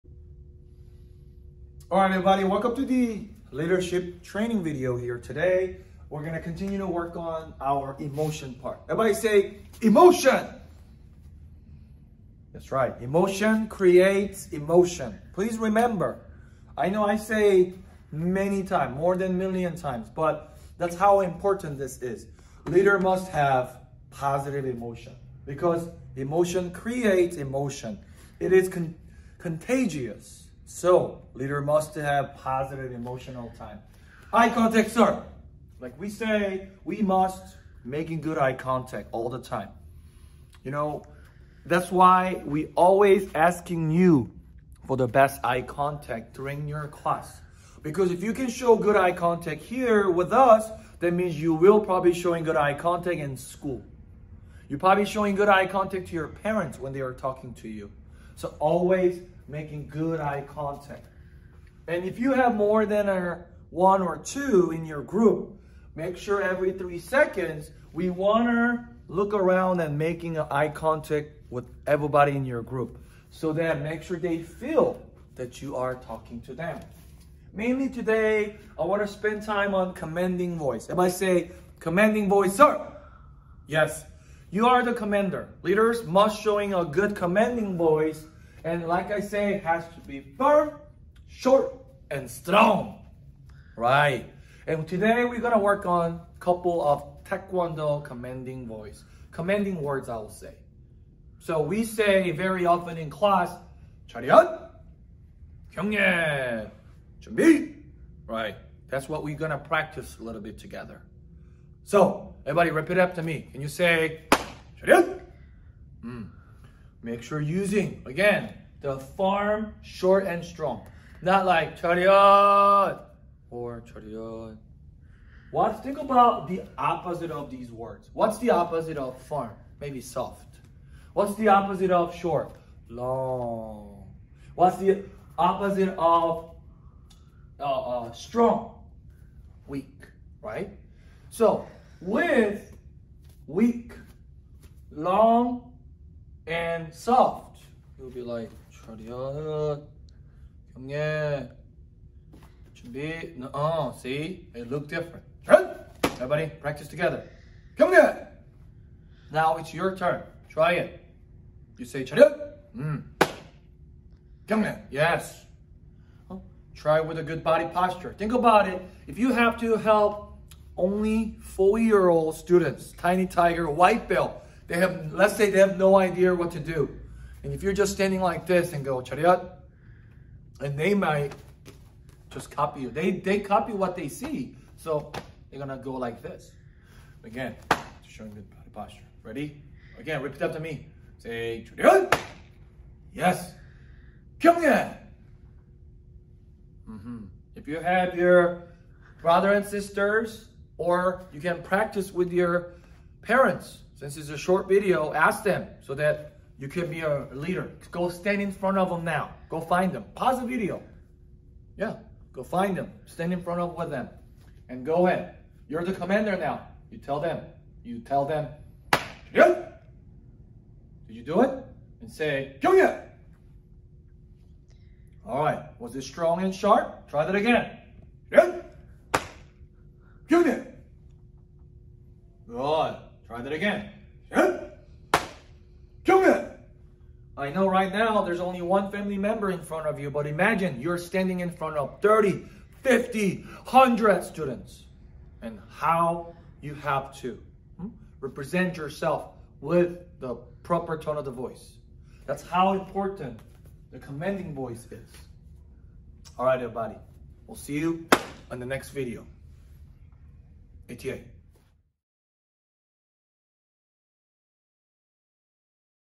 Virtual Leadership Training Lesson (Week 4-A)
Here is a Virtual Leadership Training Lesson (Week 4-A) for our Leadership Club Students and Black Belt Students,

We will post a Leadership Training Lesson video on Tuesday and a Leadership Weapon Training video on Thursday!